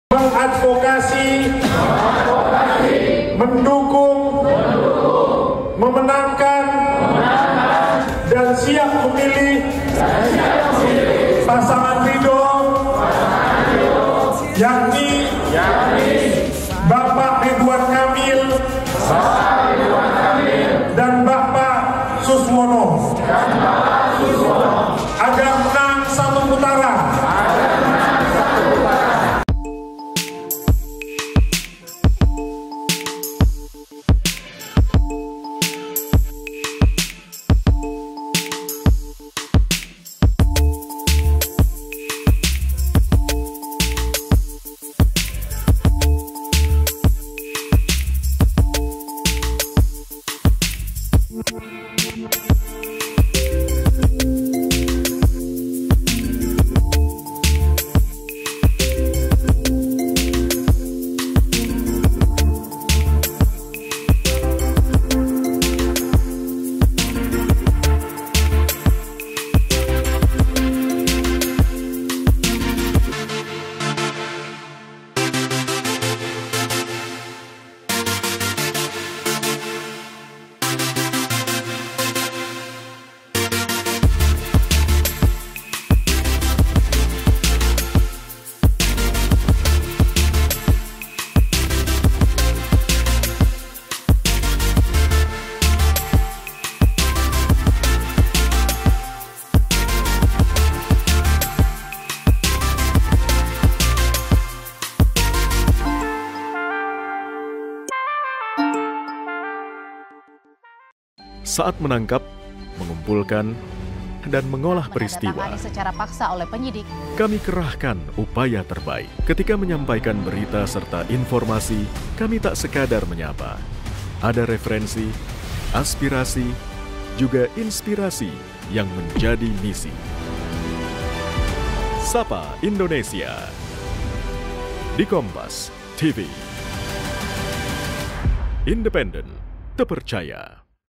Advokat GPN RI Sampaikan Ini Terkait Dukungan ke RK-Suswono
JAKARTA, KOMPAS.TV - Garda Pengacara Nasional Rakyat Indonesia (GPN RI) mendeklarasikan dukungan untuk pasangan calon gubernur dan wakil gubernur Jakarta Ridwan Kamil-Suswono di Pilkada Jakarta.

Deklarasi bertepatan dengan perayaan Sumpah Pemuda pada 28 Oktober 2024 di Ballroom Mercure Ancol, Jakarta.

Ketua Umum GPN RI Faizal Hafied mengatakan, dukungan ini diharapkan pasangan Ridwan Kamil dan Suswono (RIDO) dapat menghadirkan Jakarta Maju, Jakarta baru yang penuh rasa keadilan bagi warganya.

"GPN RI siap mendukung penuh pasangan Ridwan Kamil - Suswono untuk mengawal permasalahan hukum masyarakat Jakarta. Dan mendampingi Ridwan Kamil-Suswono dari awal pemilihan hingga di Mahkamah Konstitusi. GPN RI hadir secara totalitas untuk mengawal Kemenangan satu putaran," kata Faizal. (*)

Kompas TV
Independen | Terpercaya